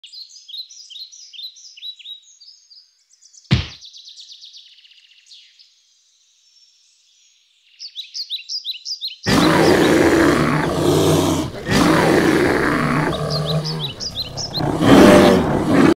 Oso Suave takes on Red Bull
Oso Suave, the girl bear extraordinaire, has an encounter with a yummy drink.